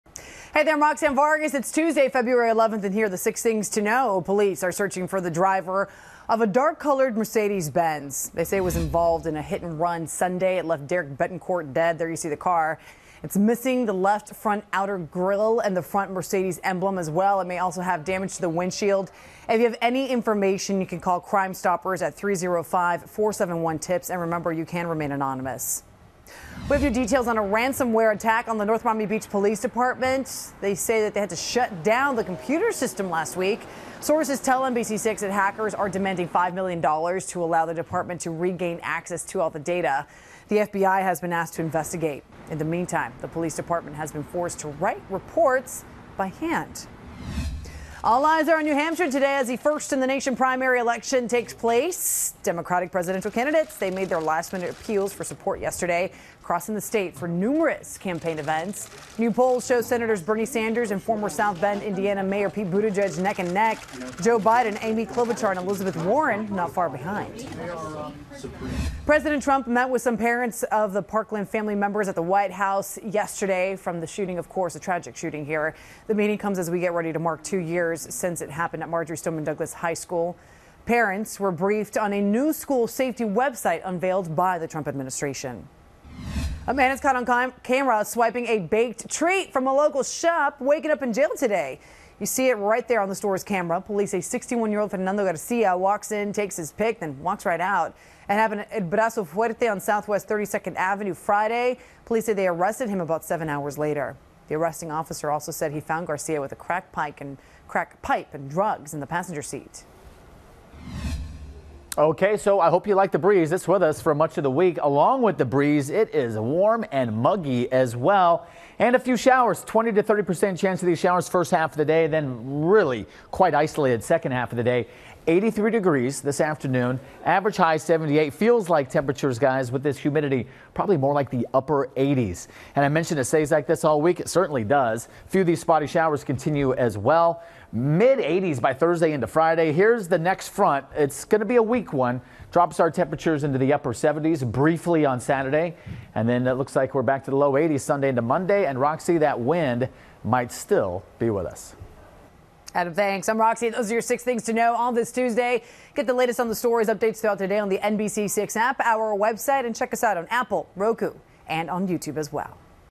Ransomware Attack Hits Local Police Force, Man 'Takes the Cake' in Robbery - 6 Things to Know
NBC 6 has the top stories you need to know for the day.